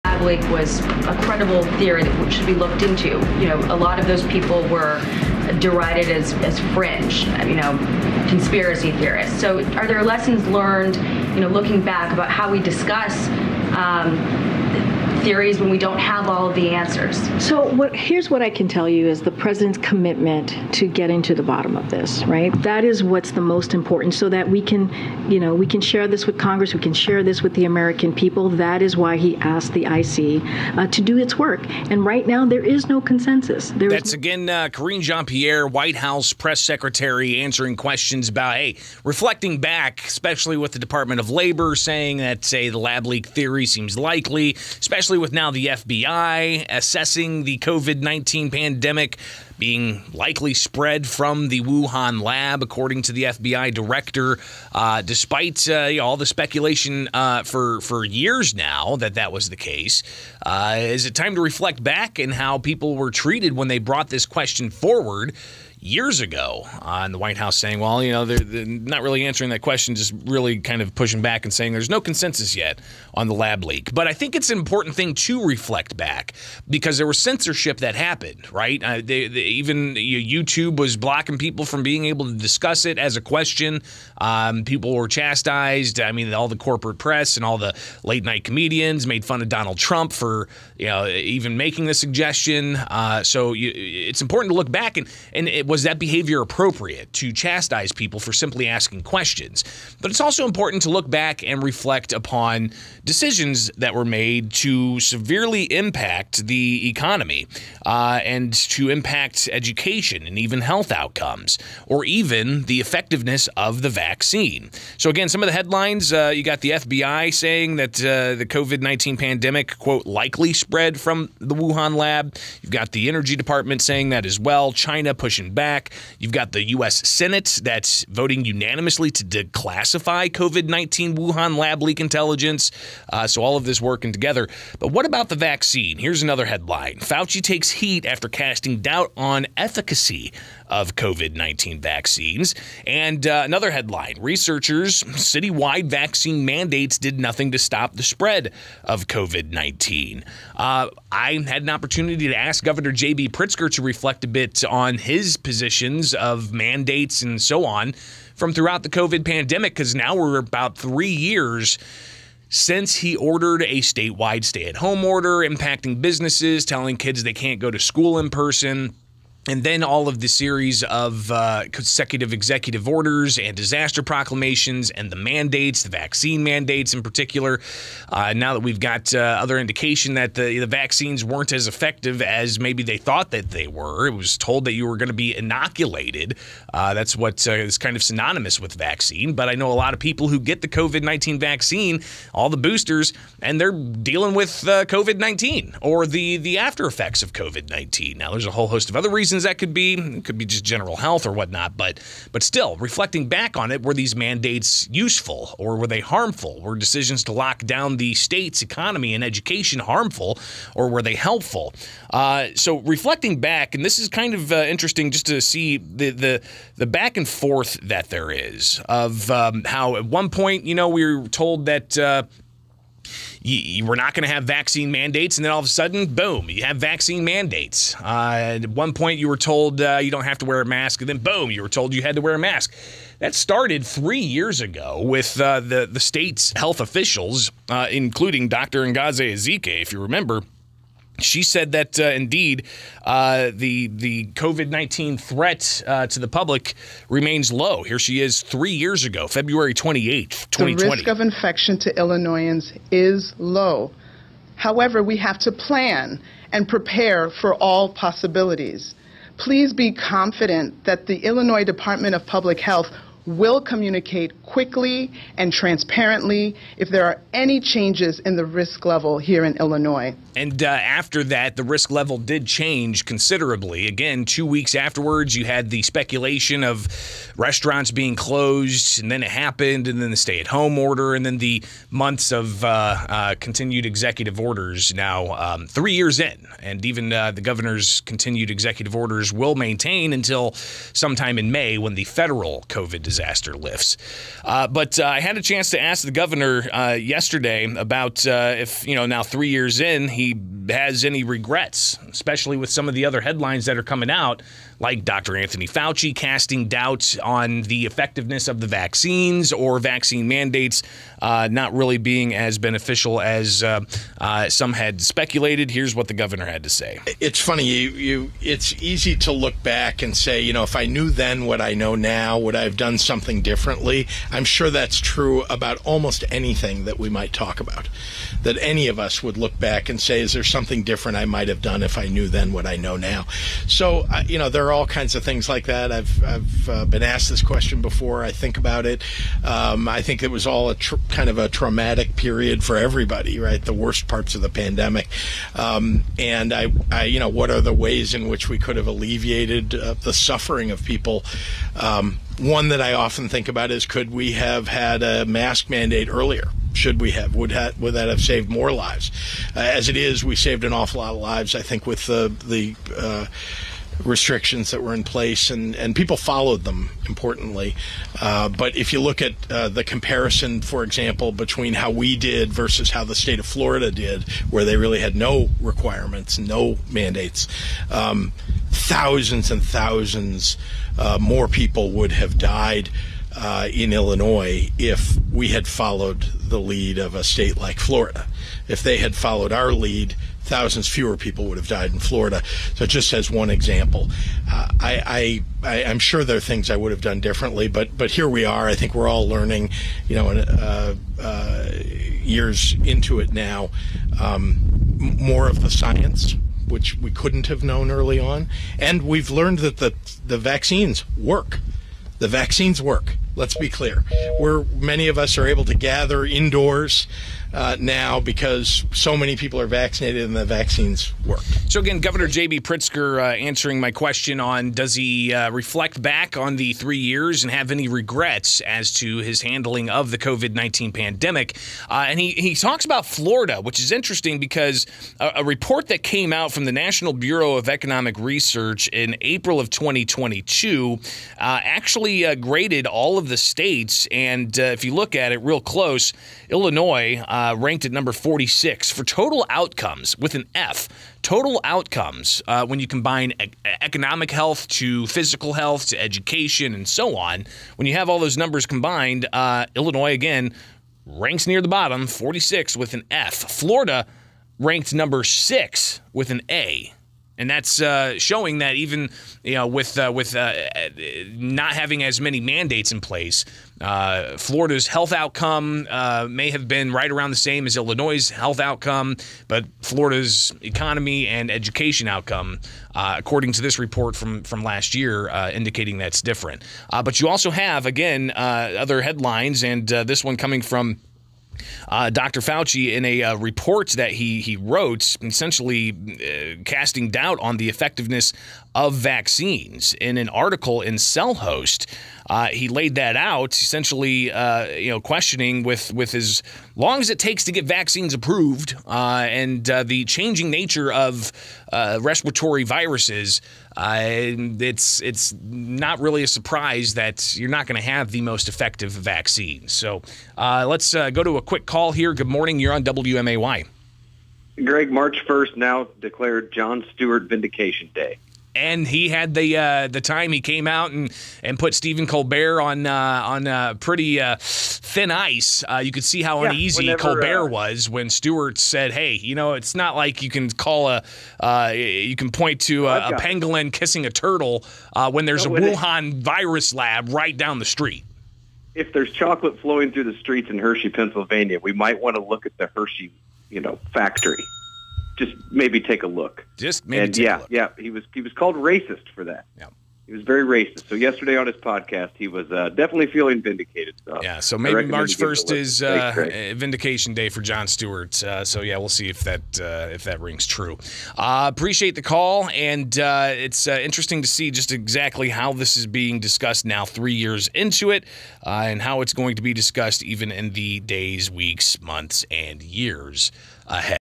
Pritzker: Florida should have followed Illinois' lead on COVID mandates and 'vaccines work'
Nearly three years into the start of Illinois Gov.  J.B. Pritzker's COVID-19 mandates, he is asked to reflect on any regrets.